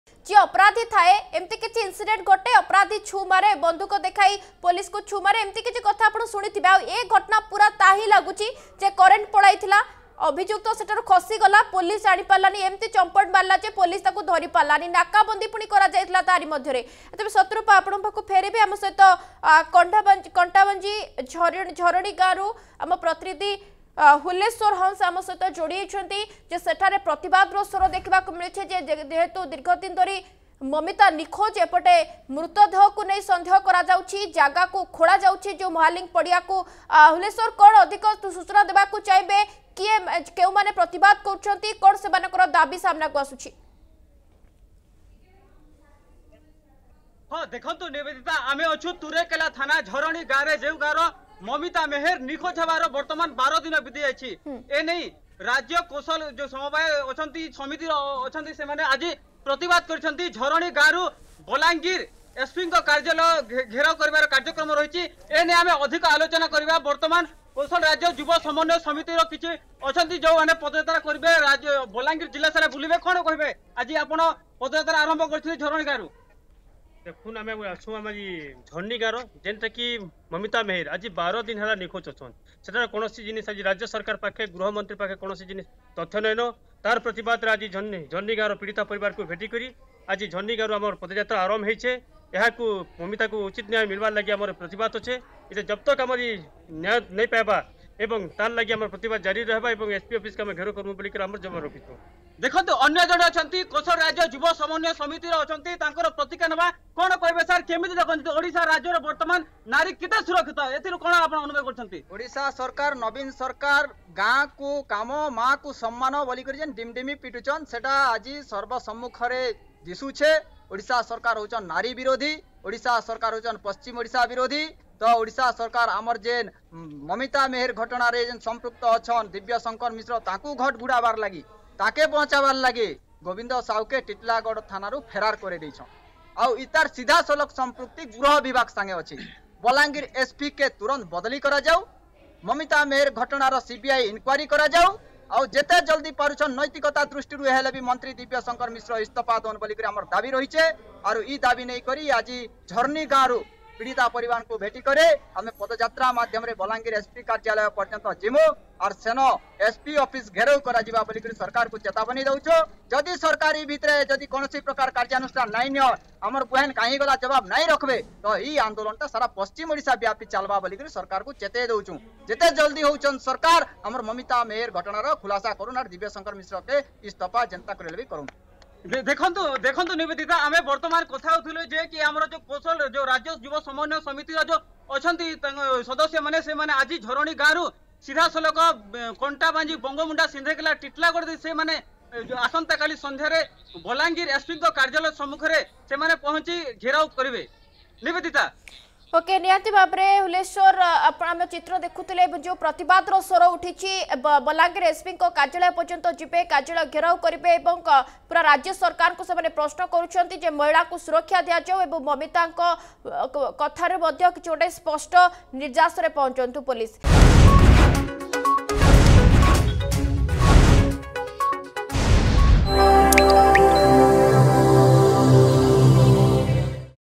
ପଡ଼ିଆରେ ମମିତାଙ୍କୁ ମାରିକି ପୋତି ଦିଆଯାଇଥିବା ସନ୍ଦେହ
ମହାଲିଙ୍ଗ ଖେଳ ପଡିଆରେ ହଲଚଲ । ପଡ଼ିଆରେ ମମିତାଙ୍କୁ ମାରିକି ପୋତି ଦିଆଯାଇଥିବା ସନ୍ଦେହ ।  ମମିତାଙ୍କ ଗାଁ ଧରଣିରେ ପ୍ରତିବାଦ । ମମିତାଙ୍କୁ ନ୍ୟାୟ ଦାବିରେ ପଦ ଯାତ୍ରା ।